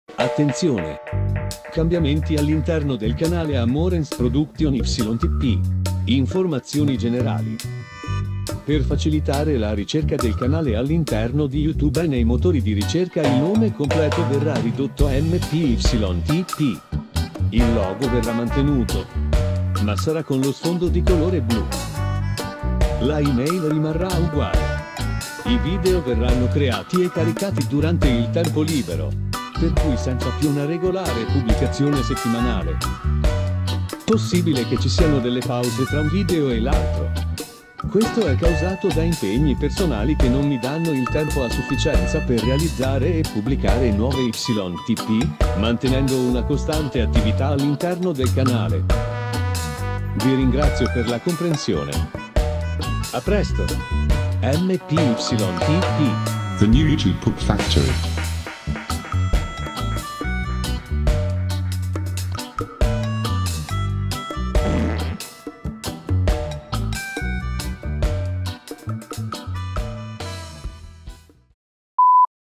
ATTENZIONE - Informazione canale [MP YTP]
ATTENZIONE

Cambiamenti all'interno del canale A MORENS PRODUCTION YTP

INFORMAZIONI GENERALI

Per facilitare la ricerca del canale all'interno di YouTube e nei motori di ricerca.

Il nome completo verrà ridotto a MP YTP.

Il logo verrà mantenuto, ma sarà con lo sfondo di colore blu.
La e-mail rimarrà uguale.

I video verranno creati e caricati durante il tempo libero, per cui senza più una regolare pubblicazione settimanale.

Possibile che ci siano delle pause tra un video e l'altro.

Questo è causato da impegni personali che non mi danno il tempo a sufficienza per realizzare e pubblicare nuove YTP mantenendo una costante attività all'interno del canale.

Vi ringrazio per la comprensione.

A presto!

MP YTP

THE NEW YOUTUBE POOP FACTORY